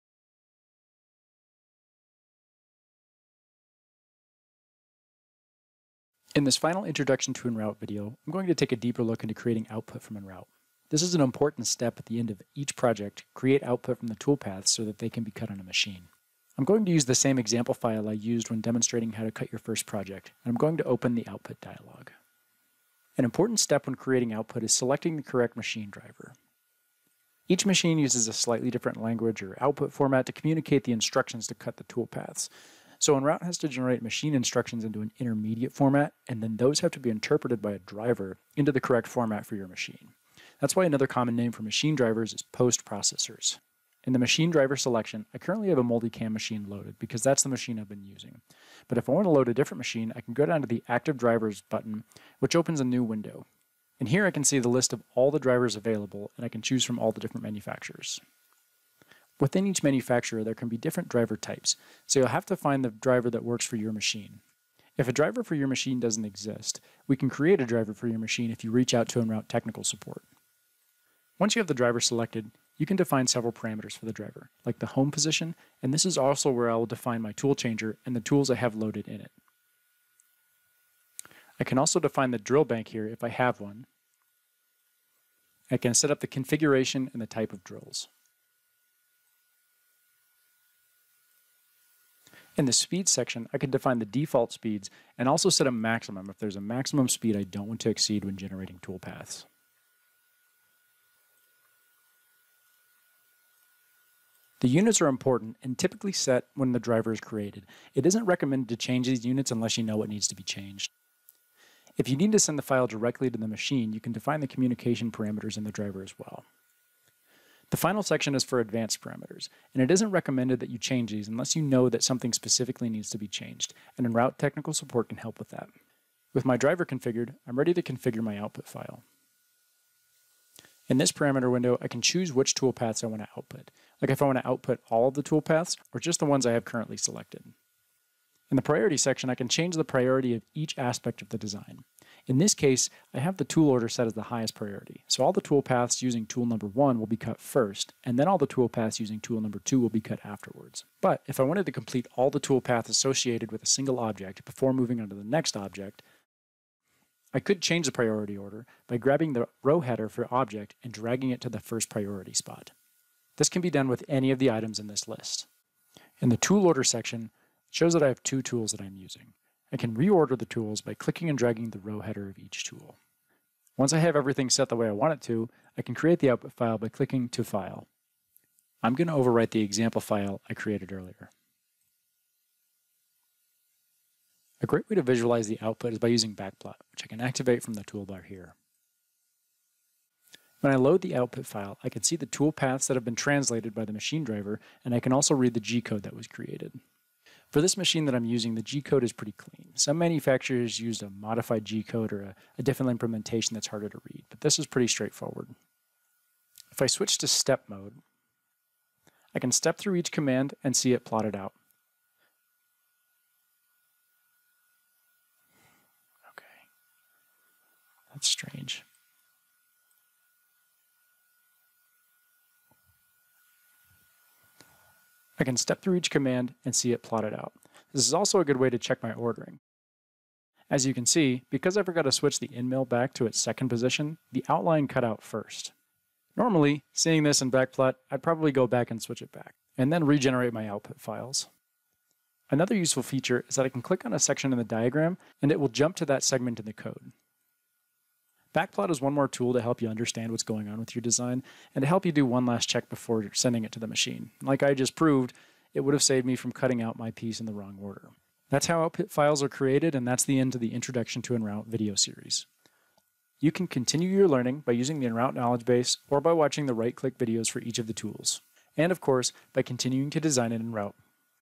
Introduction - Output
Configure the machine driver and toolpath priorities to create an output file.